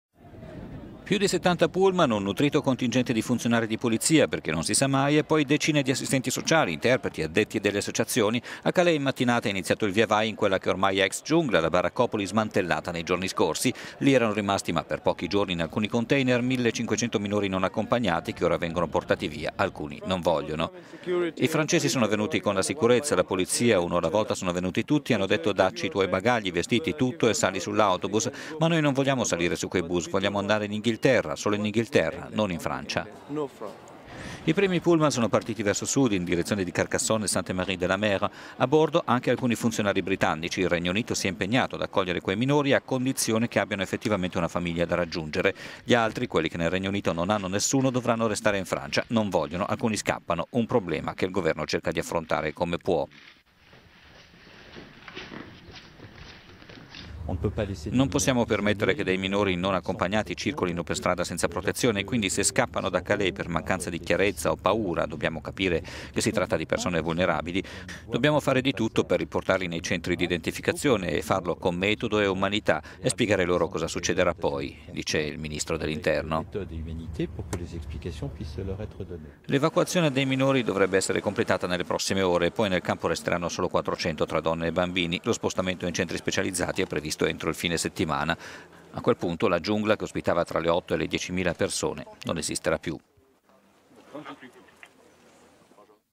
Migranti: a Calais, evacuazione per 1.500 minori - world
Più di 70 pullman, un nutrito contingente di funzionari di polizia, perché non si sa mai, e poi decine di assistenti sociali, interpreti, addetti delle associazioni: a Calais in mattinata è iniziato il via-vai in quella che è ormai ex "giungla", la baraccopoli smantellata nei giorni scorsi.  Lì erano rimasti, ma per pochi giorni in alcuni container, 1.500 minori non accompagnati, che ora vengono portati via. Alcuni non vogliono:

"I Francesi sono venuti con la sicurezza, la polizia, uno alla vo…